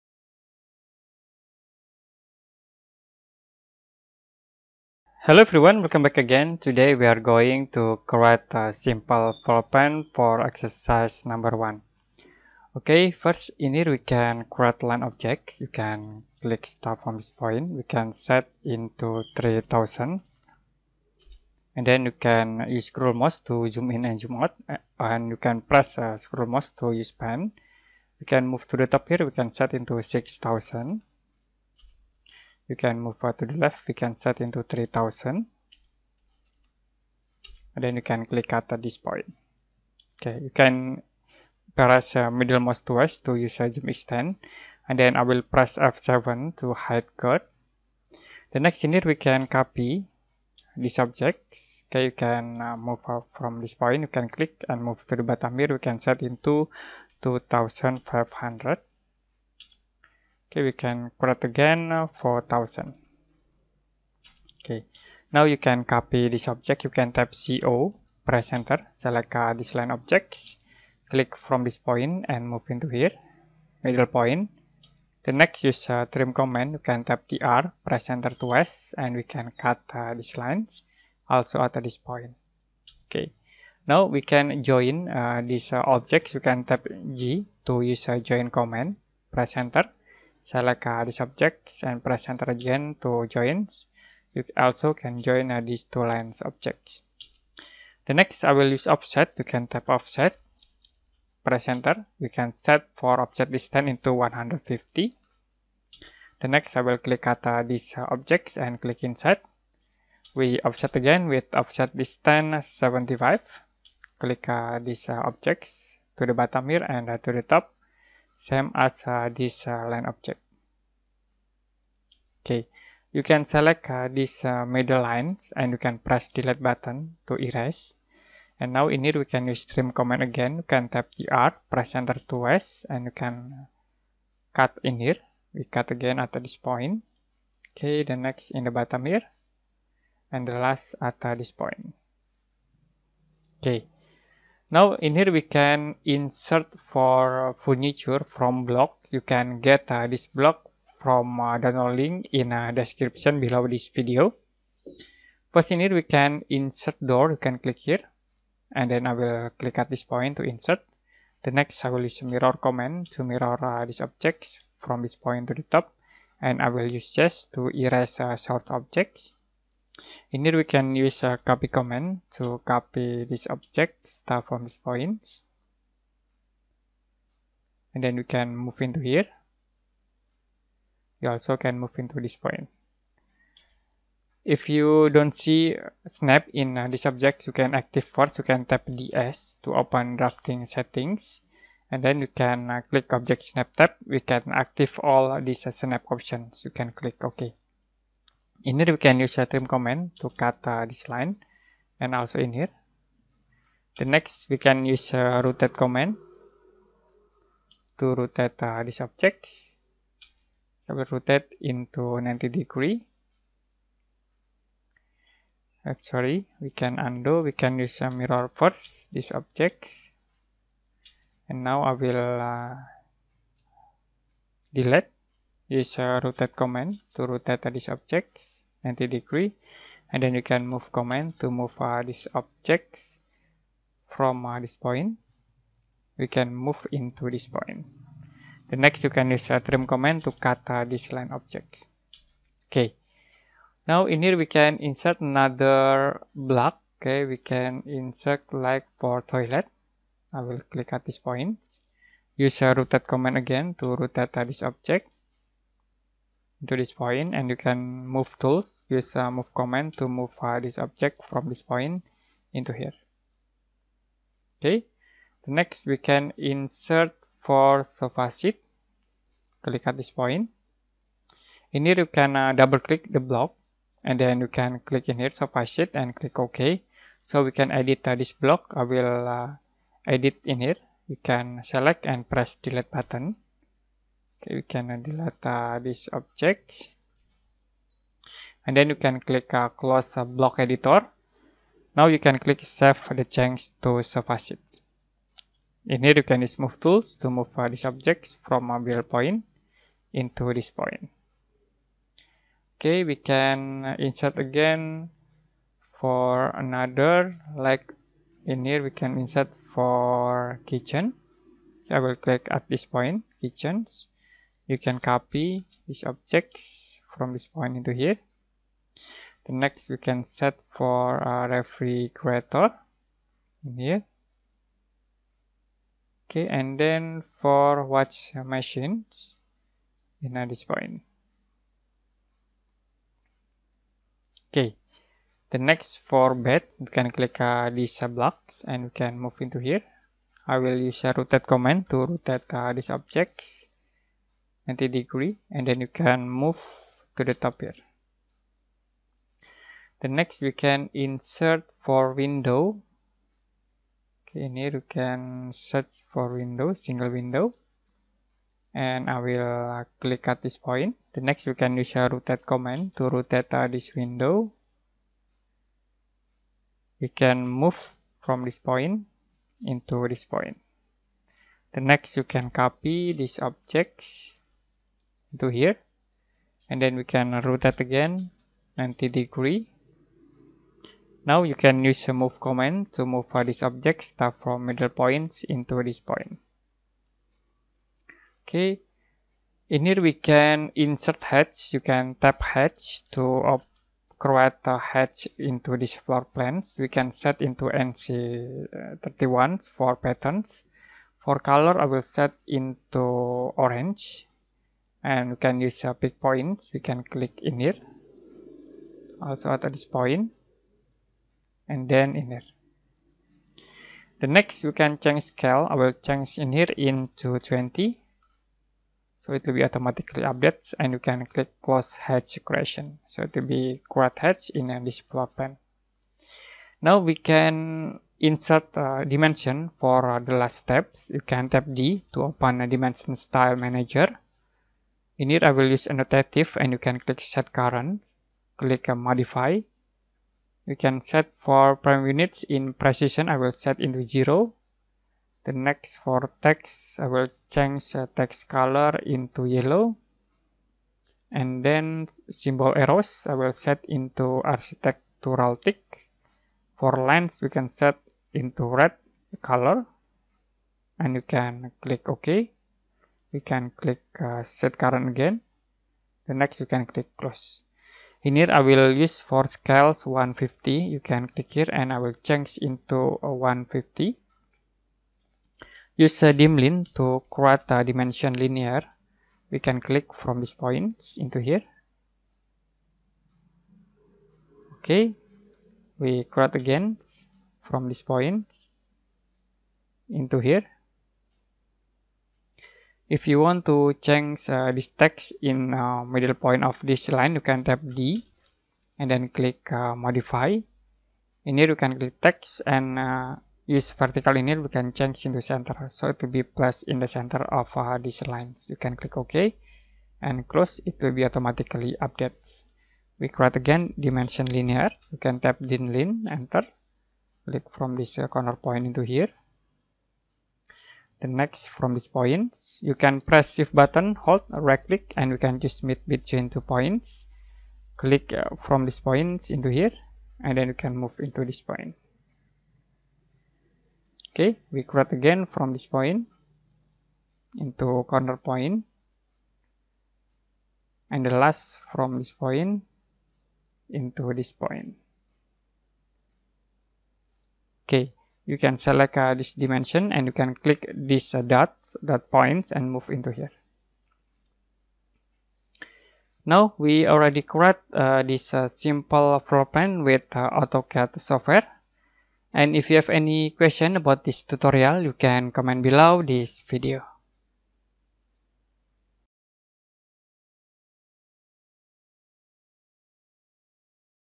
Exercise 01 AutoCAD 3x6 M Floor Plan Tutorial For Beginner
Learn autocad basic 2d floor plan tutorial for beginner with some easy command to use